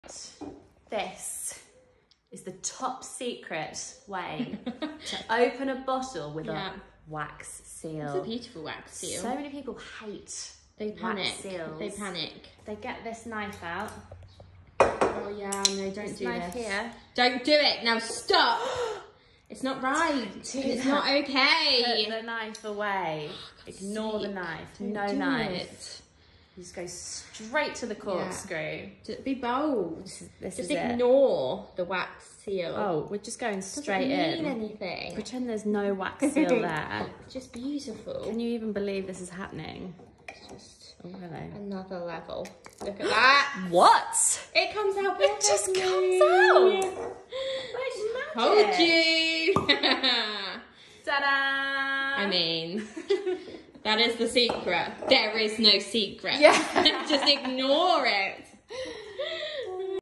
How To Open A Bottle With A Wax Seal
On today’s Elizabeth Rose Wines video, Sara Thake is back to show us how to open a bottle with a wax seal.

This is one of our own stock products

WHAT WE ARE OPENING
LONDON CRU